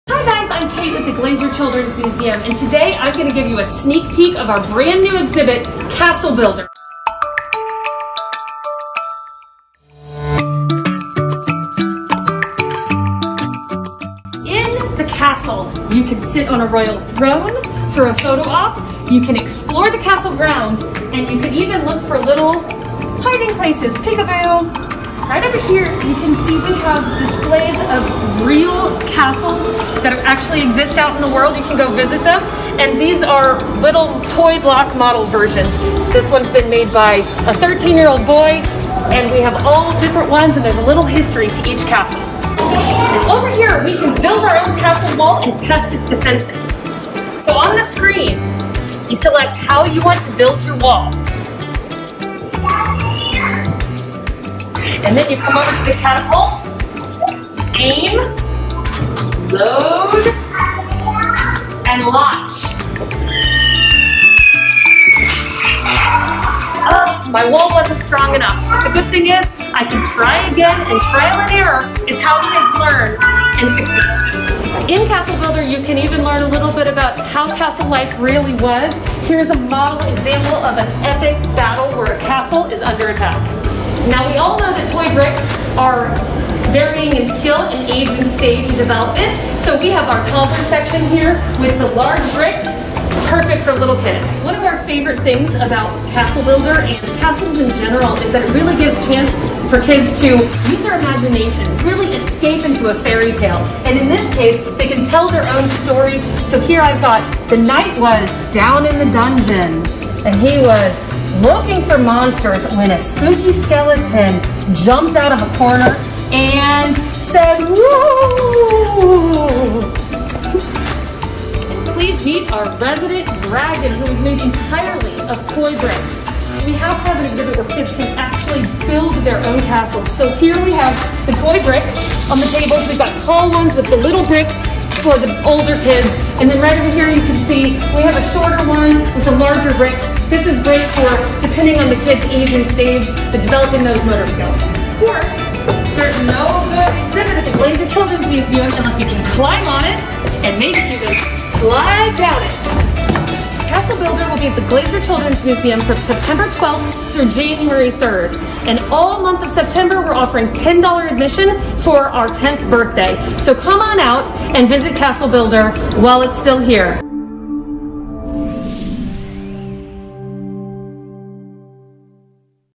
INSIDE LOOK Castle Builder at Glazer Children's Museum
A NEW exhibit is debuting this weekend!!! Head to the second floor to explore Castle Builder! It's a fairy tale, brick inspired hands on exhibit to get kids focusing on STEM and putting their imaginations to work!

Kate White, and mama of 2, shows us around the exhibit so you know what to expect before you go with the kids!

FYI: In celebration of the museum's 10th anniversary, admission is only $10 in September! Everyone ages 2 and older will need to wear a face mask, so keep this in mind as you plan your day.